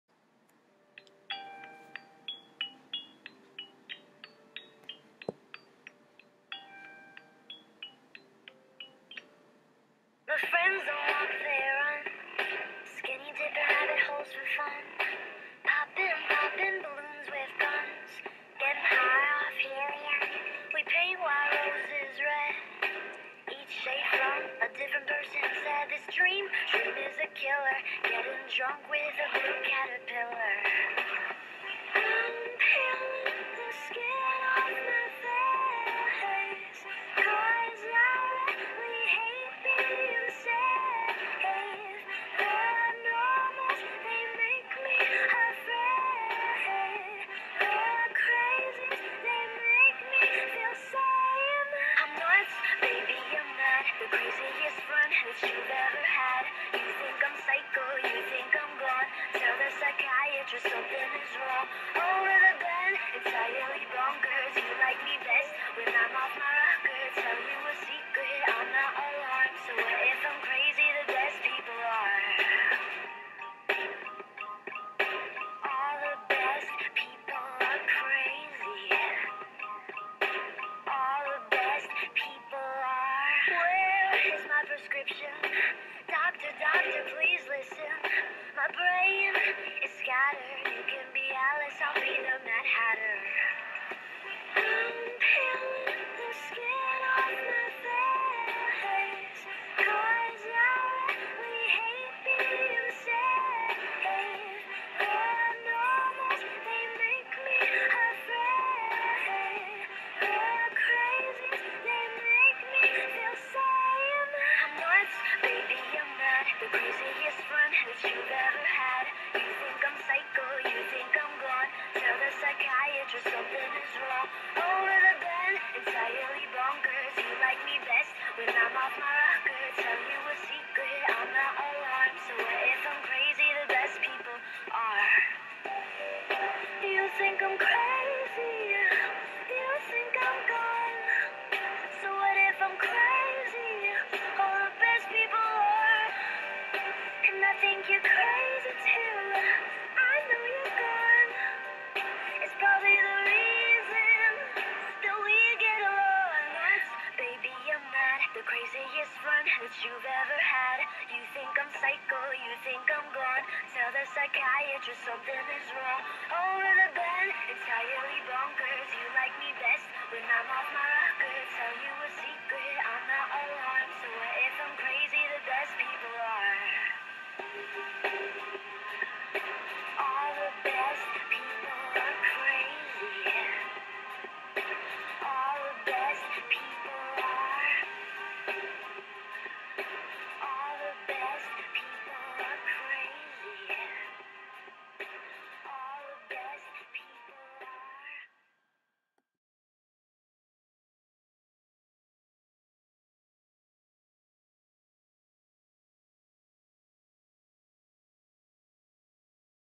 carie gore speedpaint
insatgram is @rainbowphantom2

this video was a art project asiment and i think i took ot to far but i hope this teacher gives me a fucken A for this cuz im wasting my sleep ( i really dont care i still wanted to do this ) thnnks ty for reqesting it

program sketchbook pro